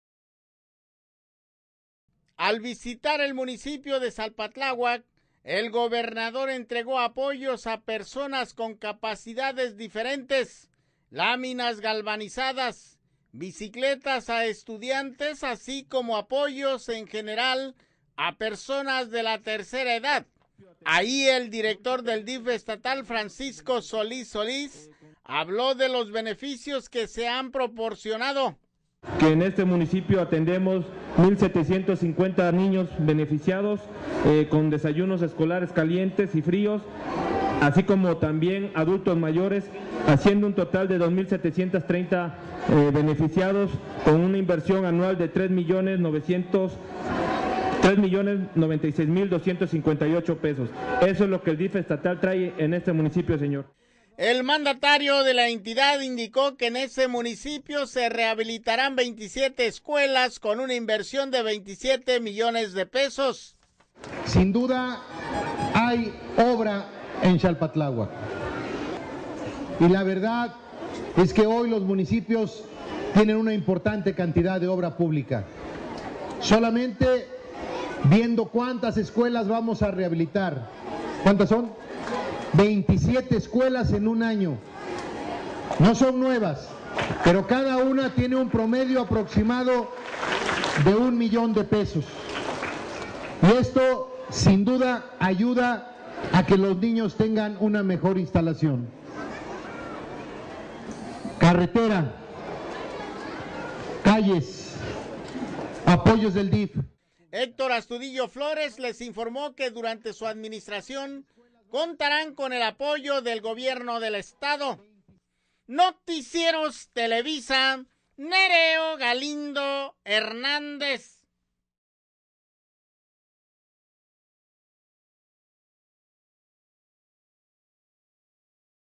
El gobernador Héctor Astudillo visita Xalpatlahuac, Gro. entrega apoyos.
El gobernador Héctor Astudillo visitó el municipio de Xalpatlahuac y entregó apoyos a personas con capacidades diferentes y apoyos en general a personas de la tercera edad.